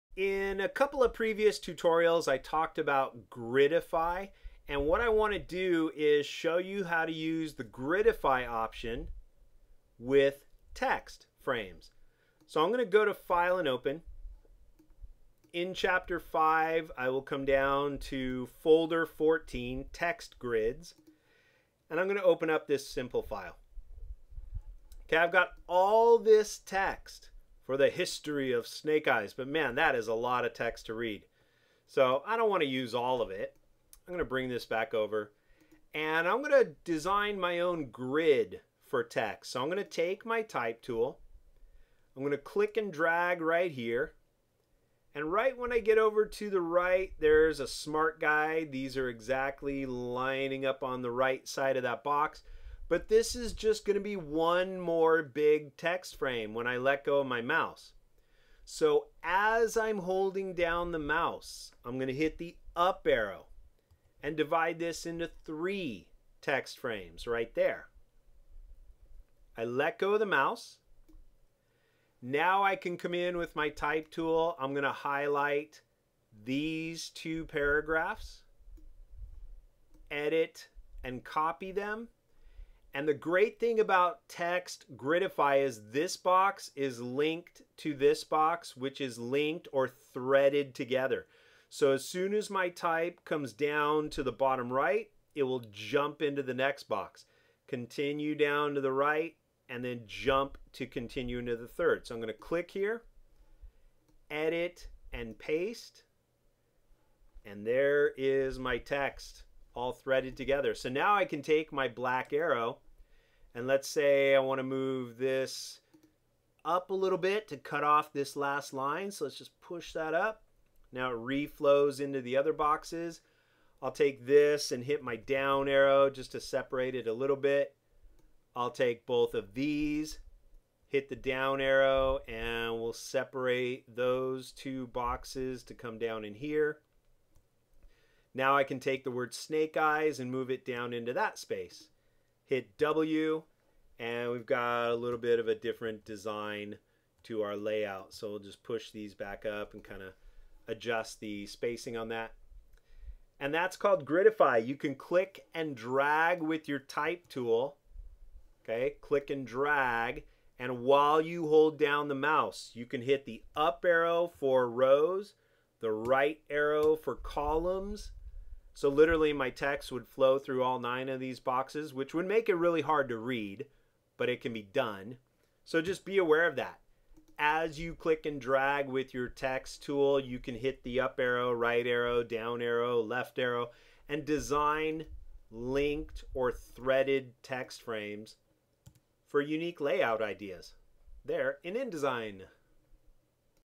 How to Gridify and Thread Text Frames in InDesign
My Tutorial on how to use the Gridify Method to lay out linked text frames in InDesign.